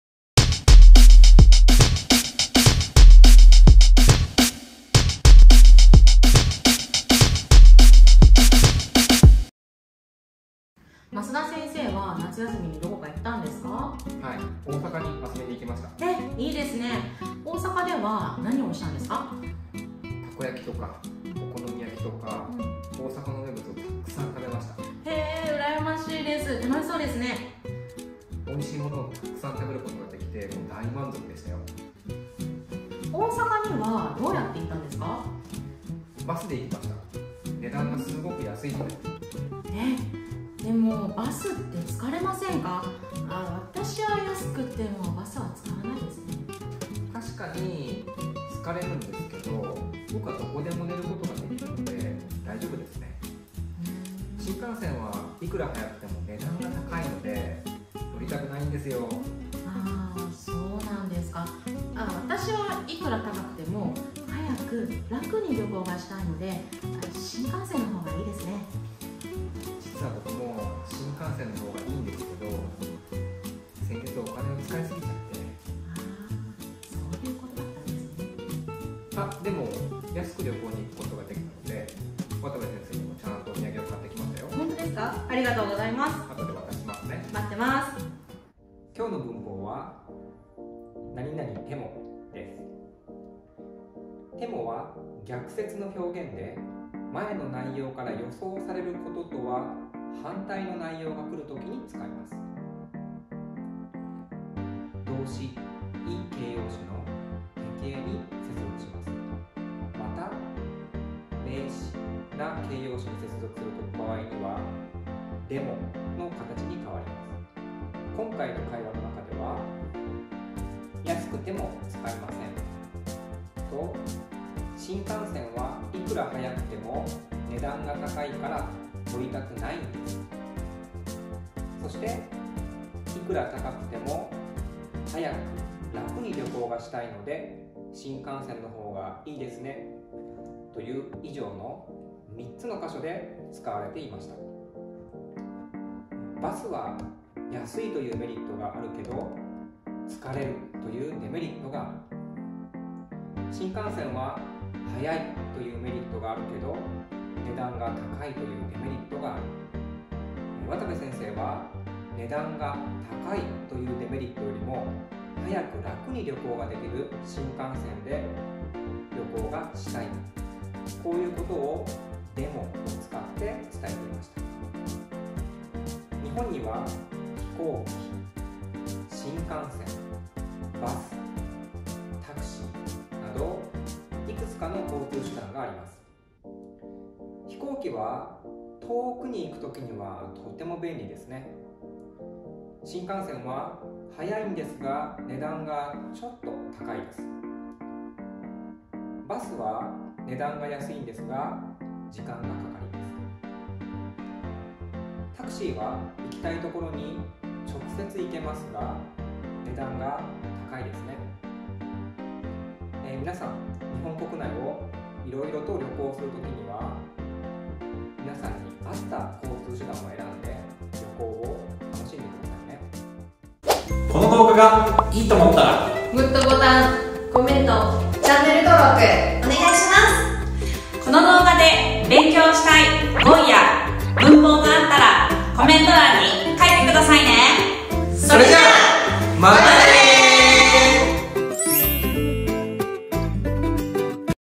【カナンの３分スタディ】N3文法 ～ても
みなさんは夏休み☀️どこか行きましたか？😊
カナンの先生👩‍🏫👨‍🏫たちも夏休みがあったので、増田先生は旅行したみたいです！✈️🌺
日本には様々な交通手段✈️🚌🚃🚕がありますが、増田先生はどの交通手段を使ったのでしょう？？😎
みなさんならスピードを取りますか？それとも金額を取りますか？？💫💰

⬇️カナン国際教育学院についての詳しい情報はこちらから⬇️